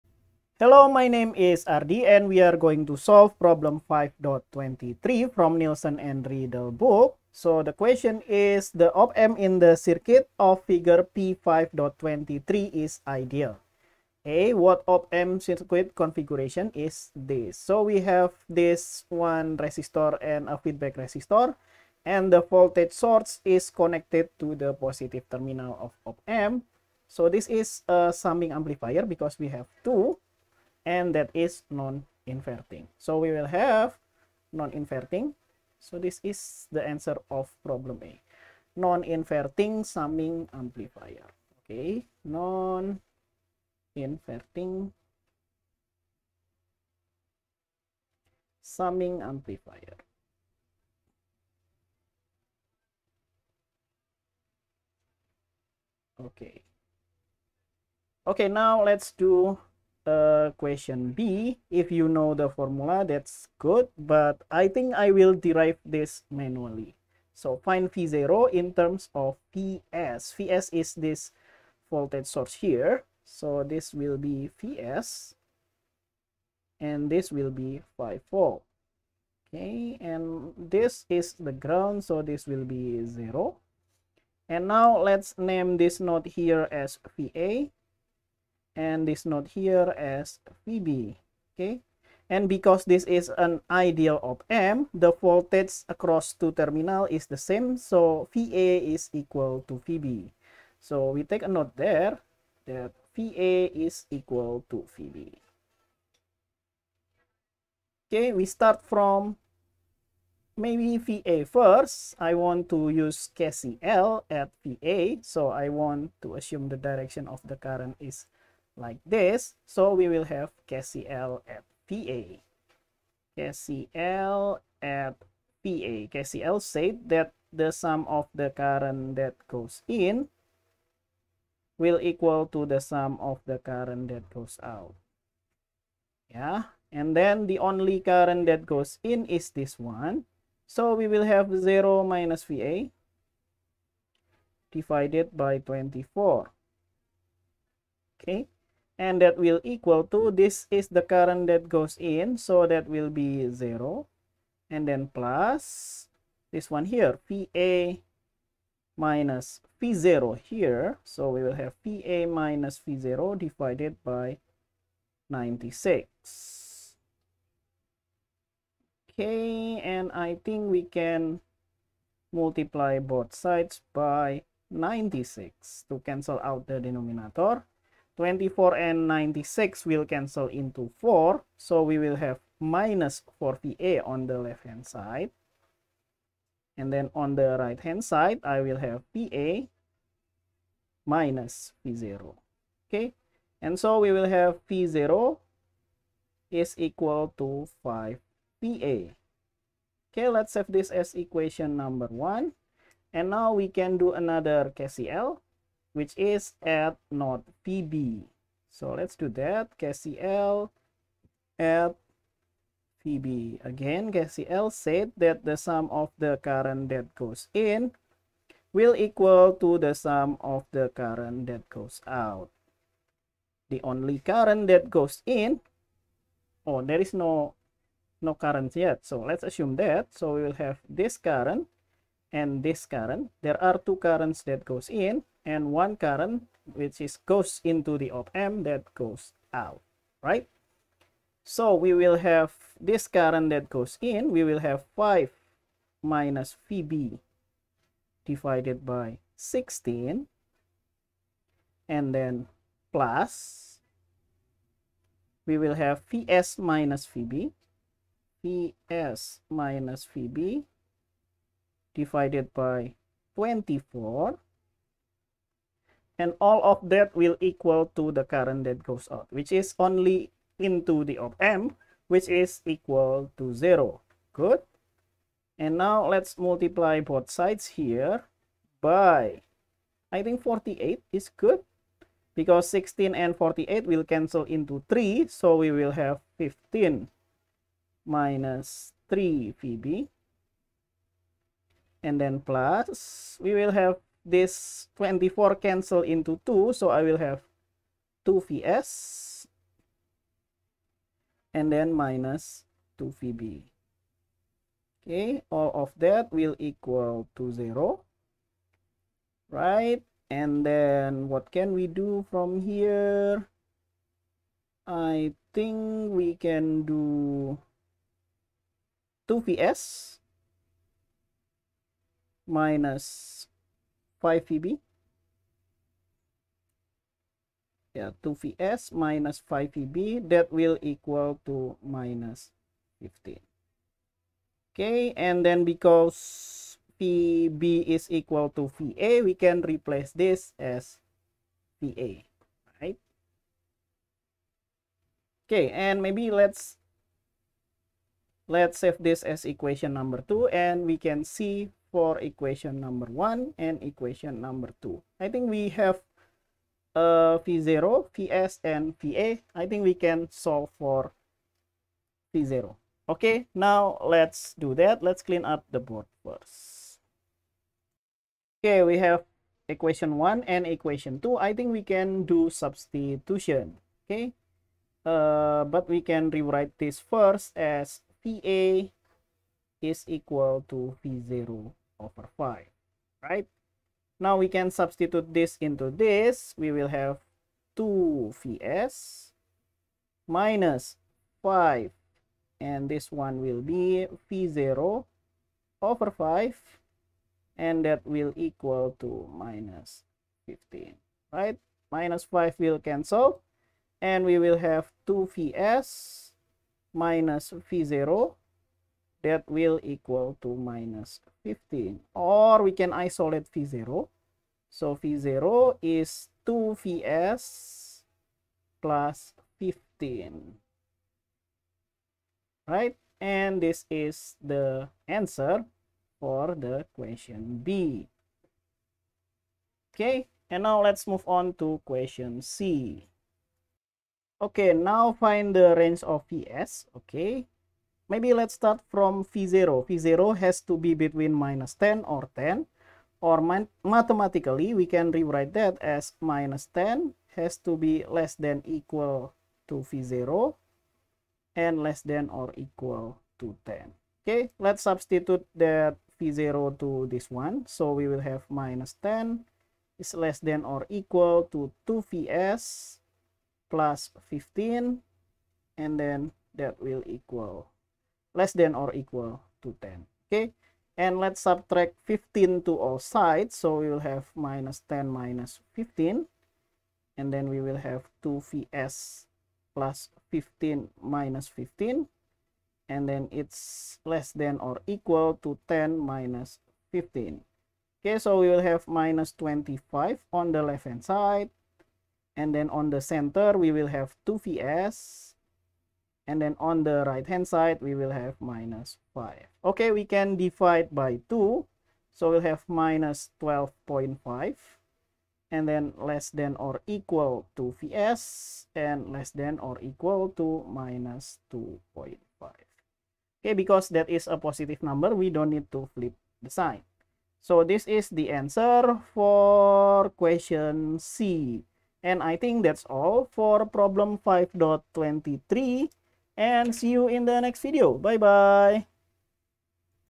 Problem 5.23 (Nilsson Riedel) Electric Circuits 10th Edition - Non-Inverting Summing Amplifier
Problem 5.23
5.23 The op amp in the circuit of Fig. P5.23 is ideal.
a) What op amp circuit configuration is this?
b) Find vo in terms of vs
c) Find the range of values for vs such that vo does not saturate and the op amp remains in its linear region of operation